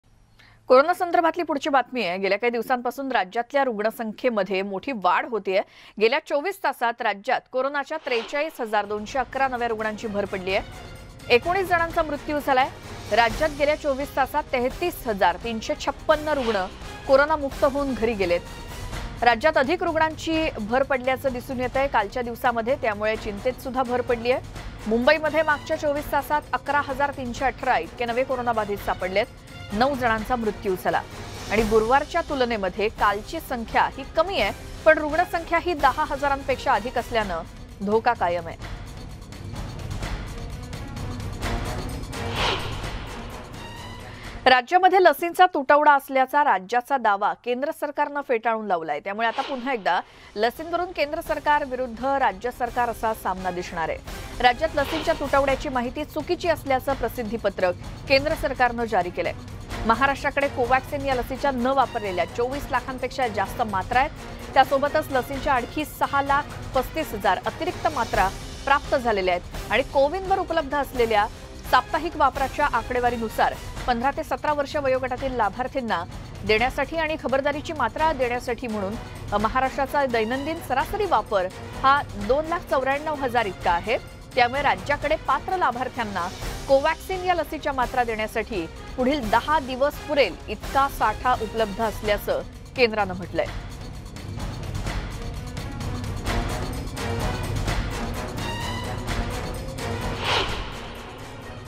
Mumbai Corona Updates | मुंबईत 11 हजार 317 रुग्णांची नोंद, दिवसभरात एकूण 9 जणांचा कोरोनामुळे मृत्यू
कोरोनाच्या नवीन प्रकार ओमिक्रॉनमुळे (Corona New Omicron) संसर्ग पुन्हा झपाट्याने वाढत आहे. ओमिक्रॉन प्रकरणांमुळे देशात (Omicron Cases in India) कोविडची तिसरी लाटही (Covid19 Third Wave) आली आहे. राज्यासह मुंबईत कोरोना रुग्णांचा आकडा वाढताच. राज्यात दिवसभरात 43 हजार 211 नवीन रुग्णांची नोंद तर मुंबईत 11 हजार 317 रुग्णांची नोंद. दिवसभरात एकूण 9 जणांचा कोरोनामुळे मृत्यू झाला आहे.